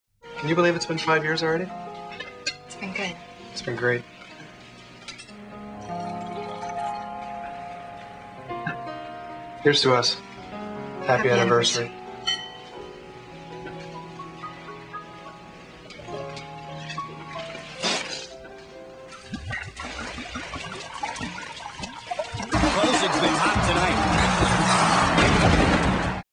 Olaf Kolzig - Romantic Dinner
Funny Washington Capitals commercial from a few years ago.